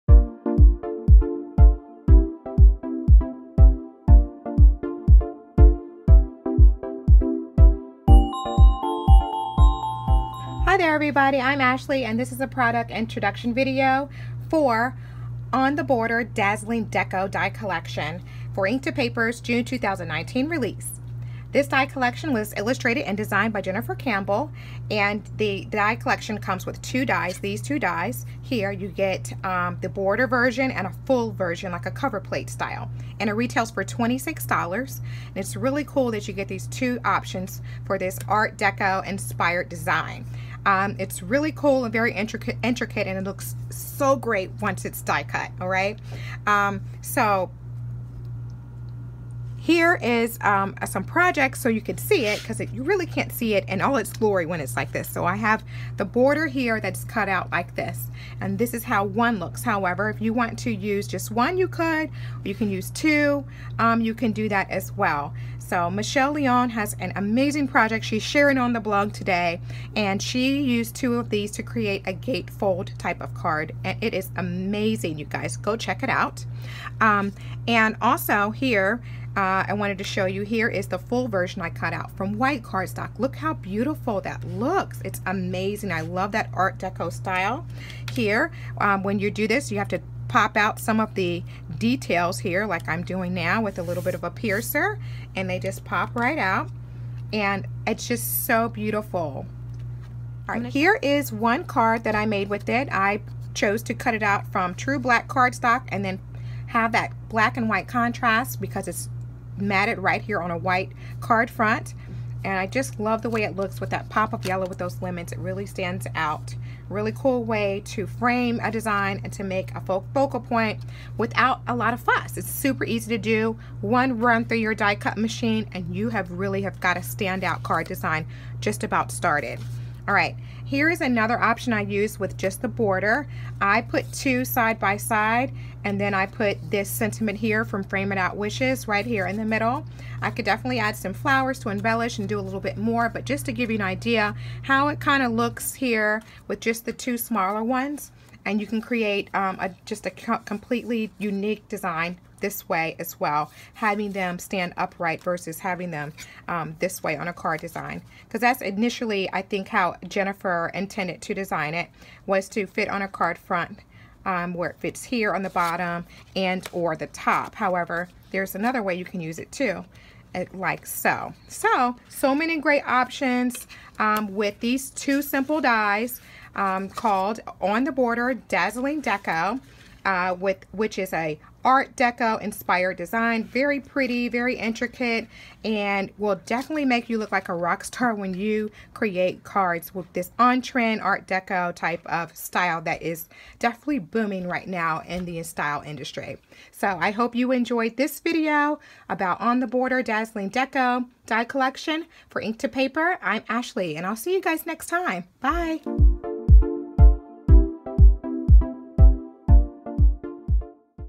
Introducing On the Border: Dazzling Deco
This video is introduces On the Border: Dazzling Deco die collection for Ink to Paper's June 2019 Release.